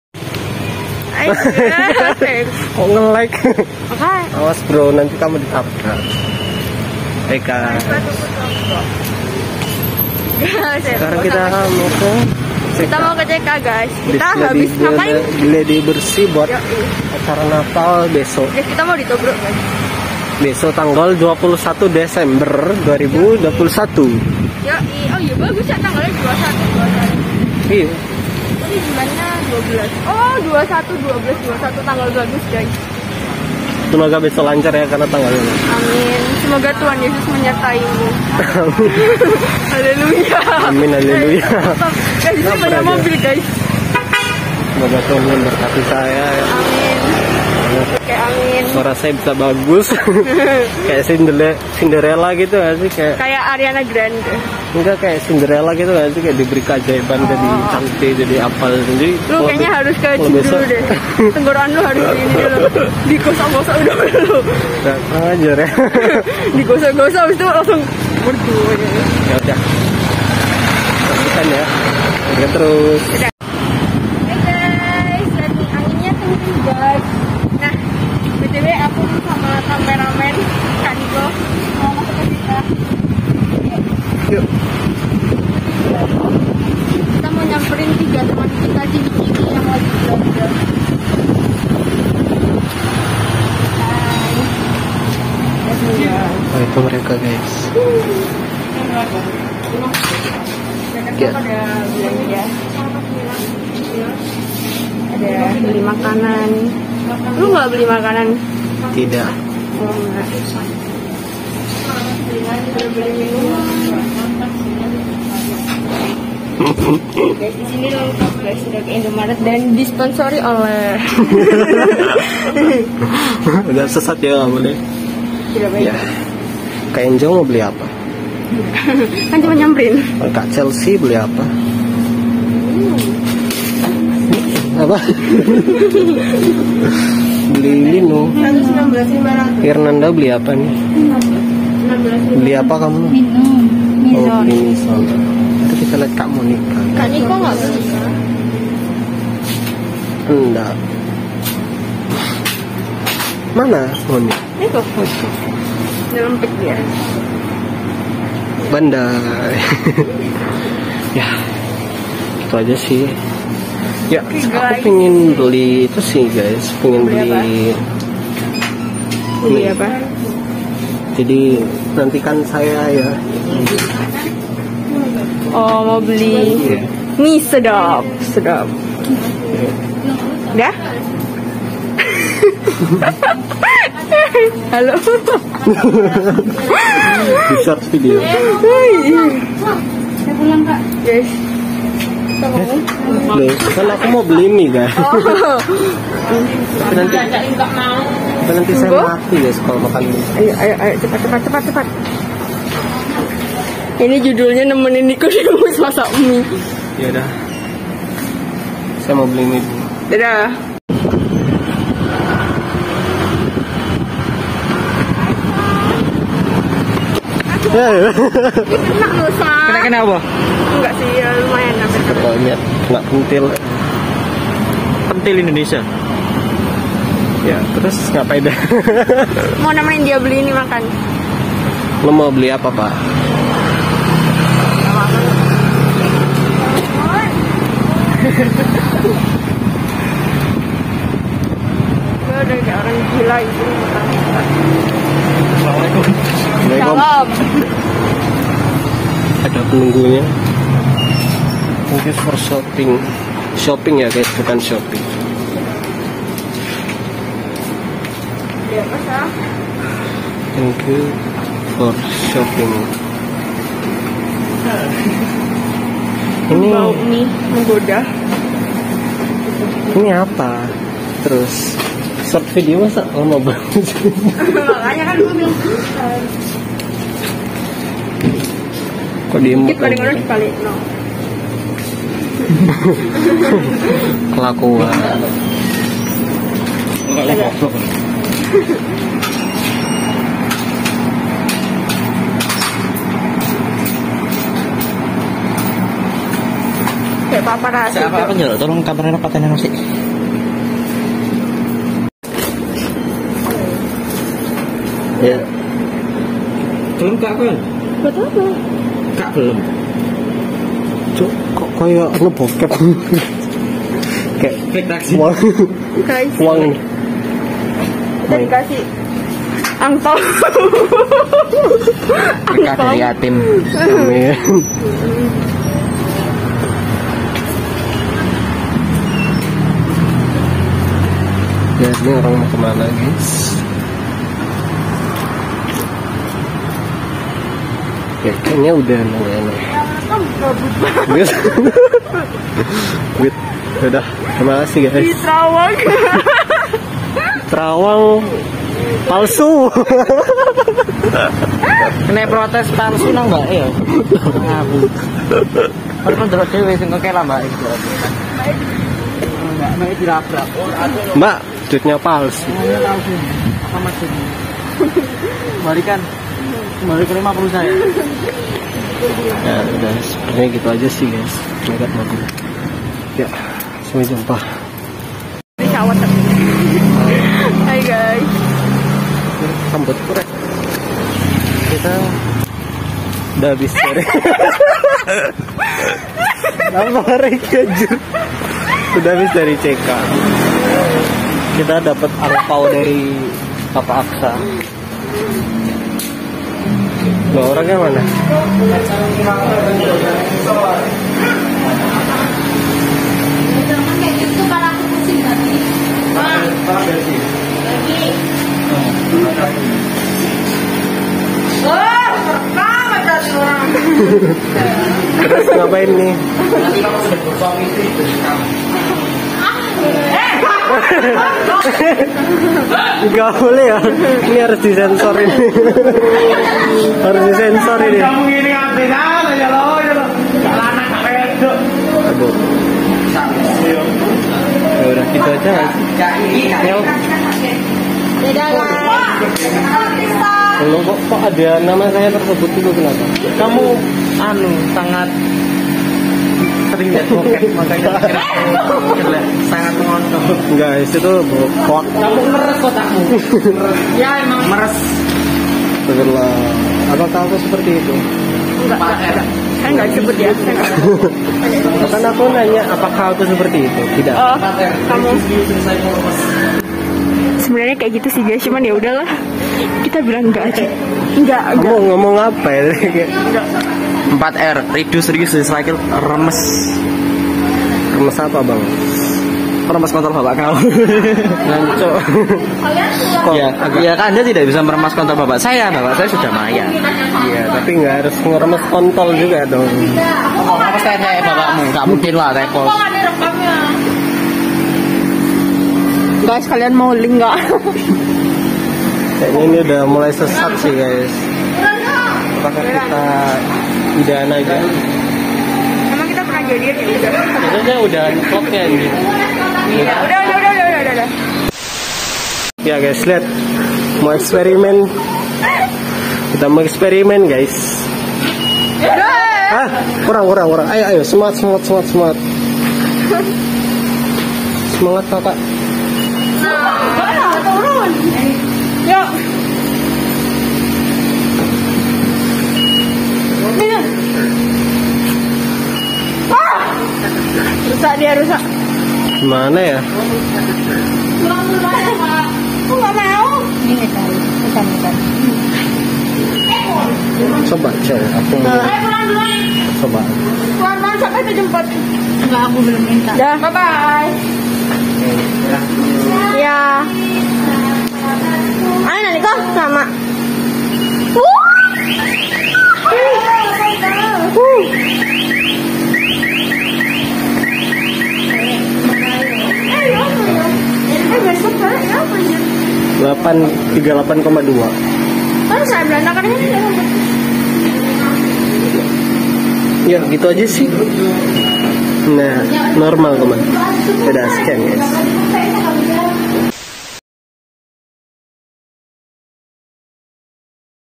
VLOG KEGABUTAN SETELAH GLADI BERSIH NATAL ✔️
Hi frenn, jumpa lagi dengan kita SRV (Sekumpulan Random Vlog) 🥰

Hai guyss..
Untuk mengisi waktu gabut kita setelah gladi bersih, kita akan mengajak kalian semua jalan ke CK wkwkwk...

Daripada penasaran, mending kalian ikuti keseruan kita yukk!! 😤